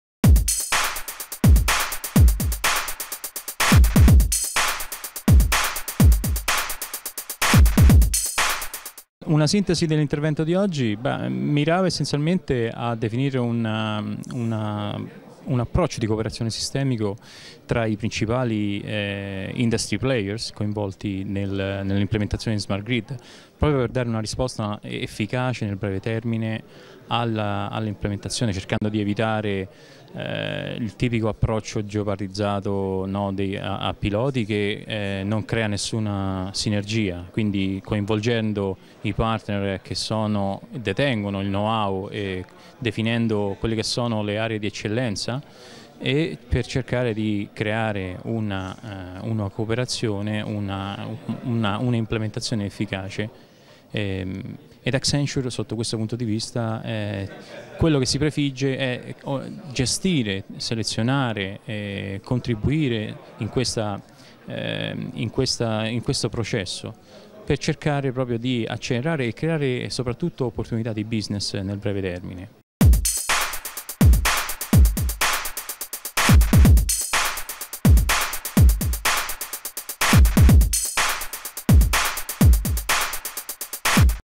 Gli industry player si raccontano
Approccio ai sistemi dei principali player in ambito smart grid la sintesi di Alfonso Bocchetti di Accenture